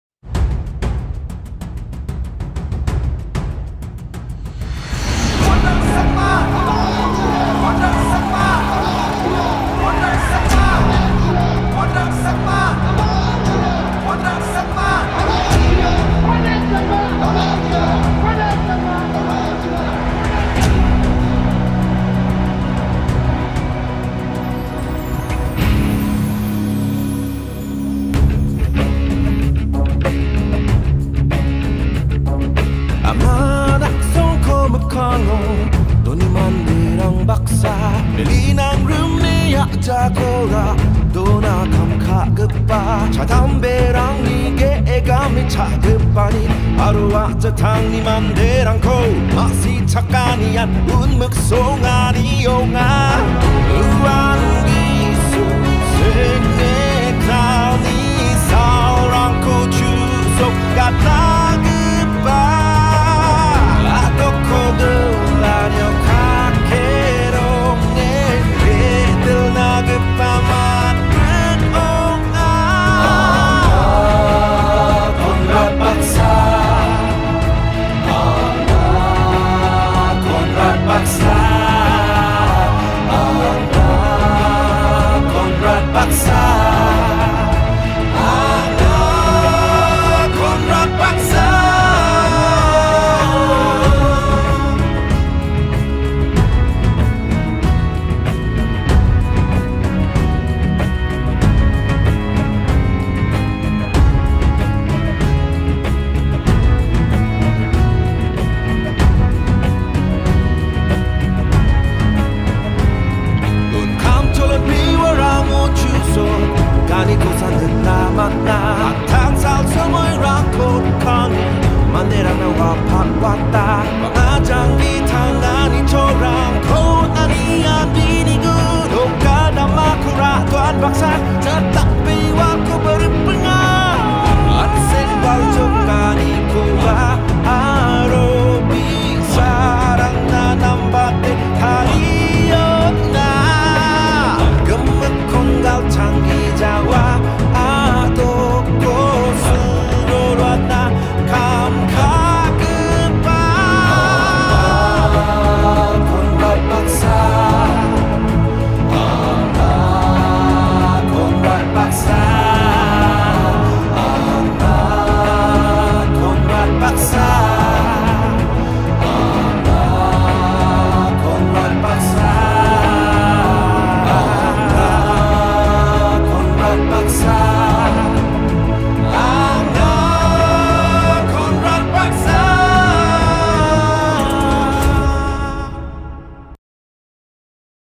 NPP CM MEETING IN ADOKGRI PLAYGROUND 2023
CM MEETING NPP
CONRAD SANGMA
OFFICIAL VIDEO
2023
IN
ADOKGRI PLAYGROUND

CAMERA MAN
PRANJIT, SUBYA, HIMANGSHU||